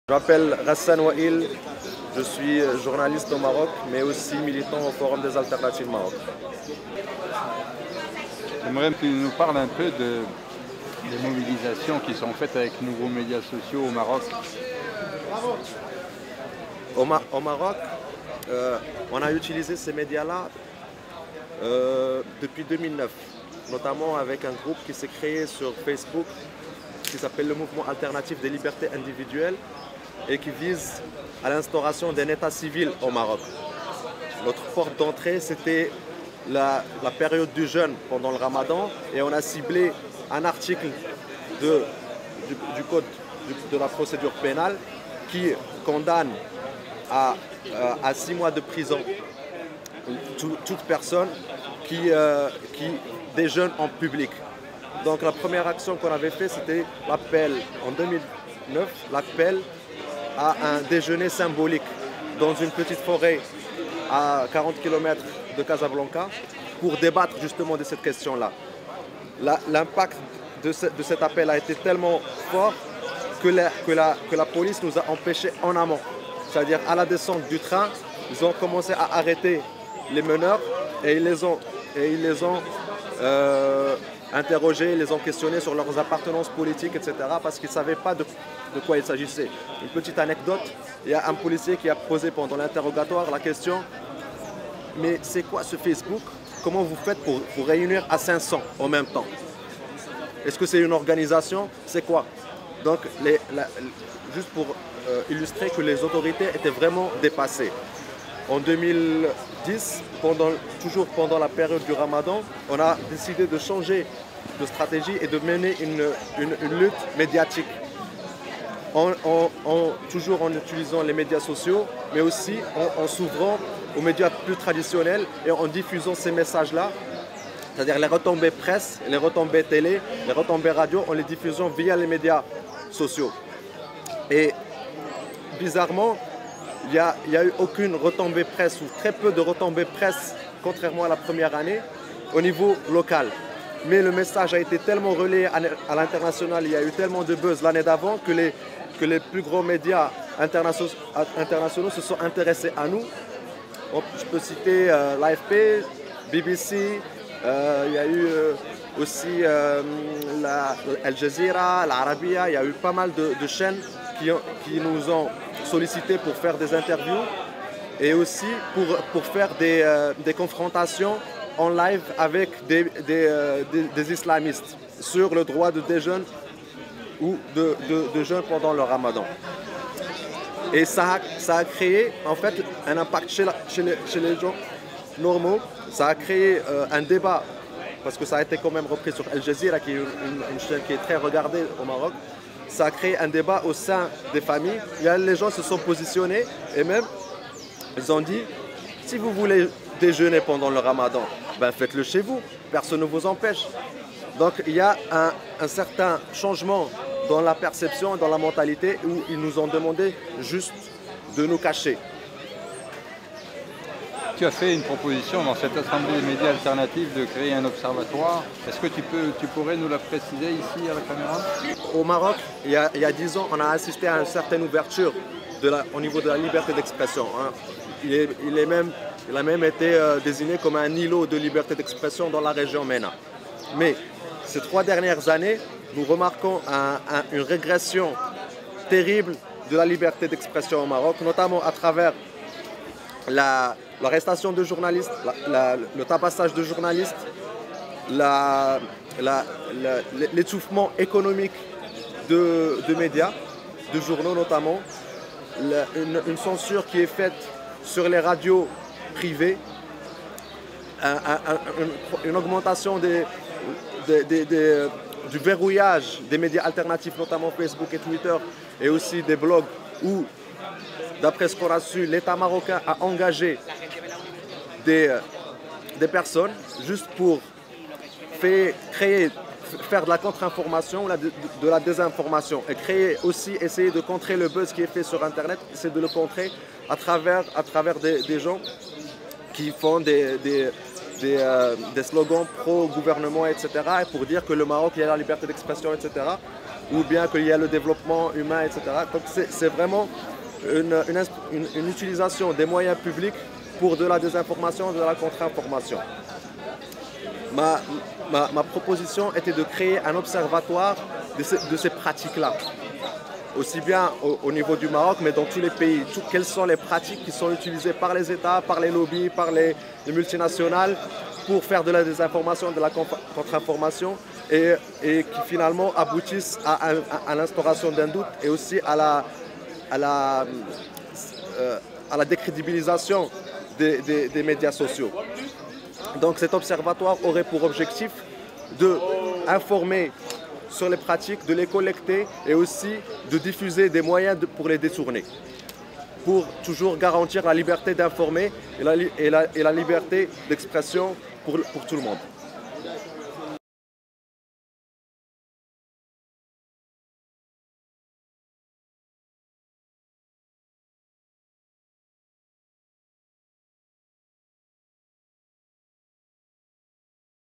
Liberté d'expression et médias sociaux au Maroc: un état des lieux
Ghassan Wail El Karmouni est journaliste et membre du Forum des Alternatives du Maroc.

Entrevue réalisée par Alain Ambrosi pour remixthecommons.org à Dakar  le 11 février 2011 lors du Forum Social Mondial.

L'assemblée sur la communication du FSM a émis le 12 février une déclaration